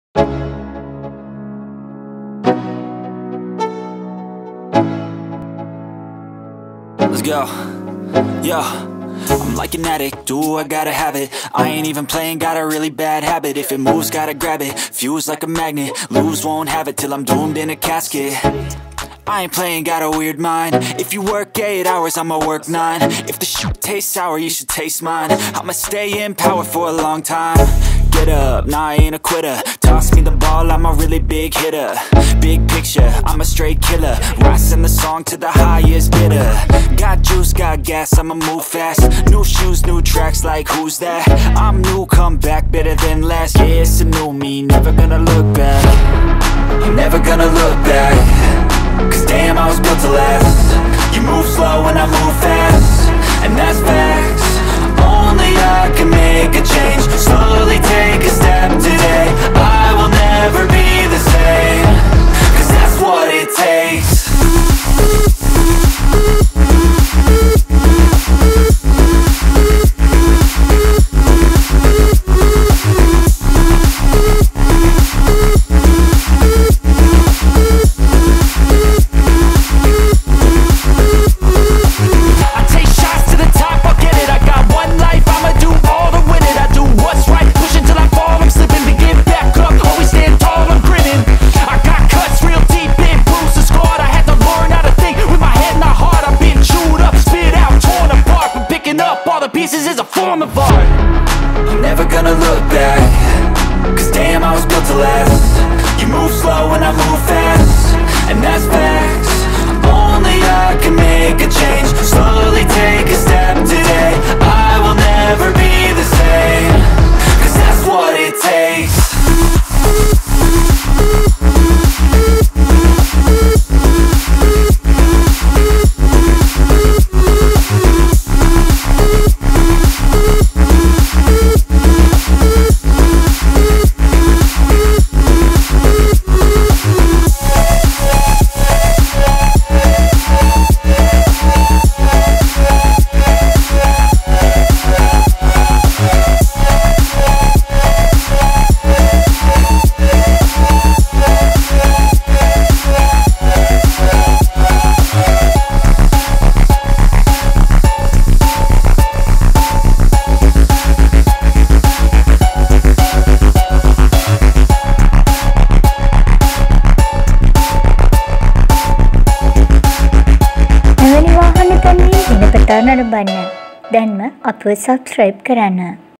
mitsubishi lancer for sale | ikman.lk wahana | used vehicle for sale
Mitshubishi lancer
CB 1
1994 brand new
modify car
26 LKS
Location- Matara